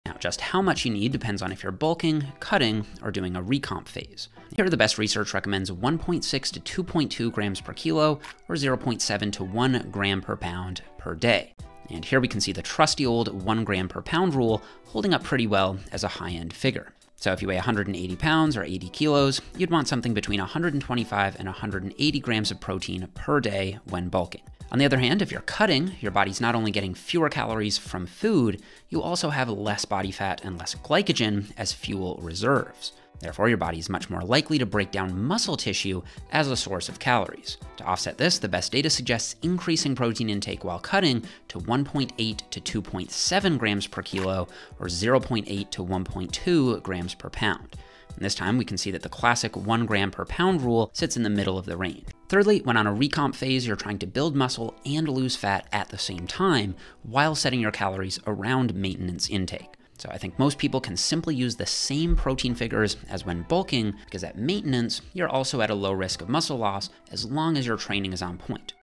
Protein Intake for MAXIMUM Muscle Growth (Nippard Science)🤯
Fitness scientist Jeff Nippard answers the most critical question in nutrition: How Much PROTEIN do you need daily for optimal muscle gain and fat loss? This clip breaks down the scientific consensus on Protein Intake and dispel common myths.

Discover the optimal range of grams of protein per pound (or kg) of body weight, whether you're a beginner or an advanced lifter. Learn about the science of protein synthesis and the role of protein for maximizing recovery and body recomposition.

Stop wasting your gains! Apply the science from this clip to your diet today. Follow @EliteHumanLab for more Fitness Science and evidence-based Training Tips!